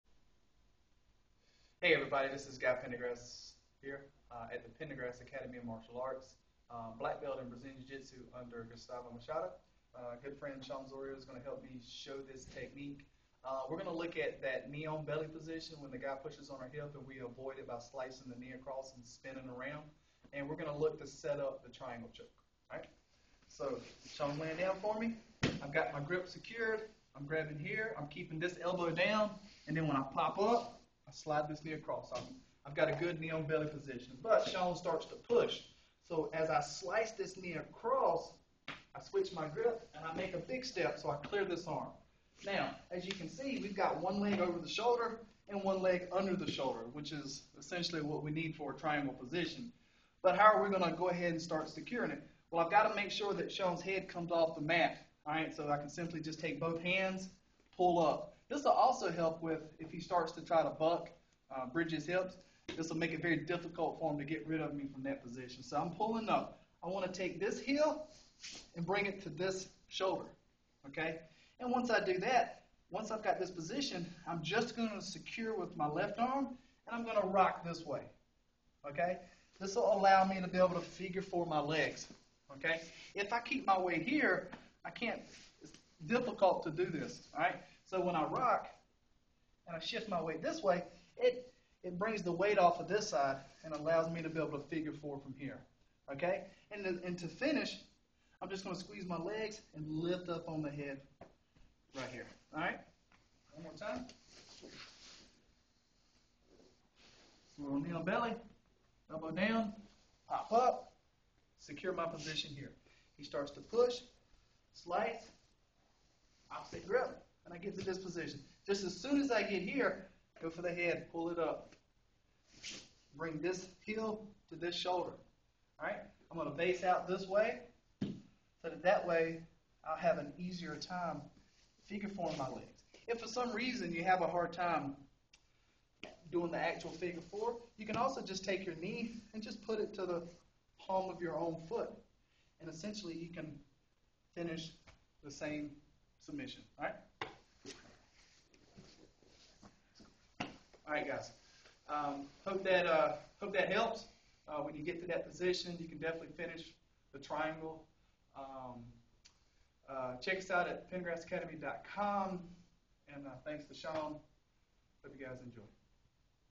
Triangle Off Knee on Belly Spin
Guy Pendergrass demonstrating a triangle set up off of the knee on bell spin.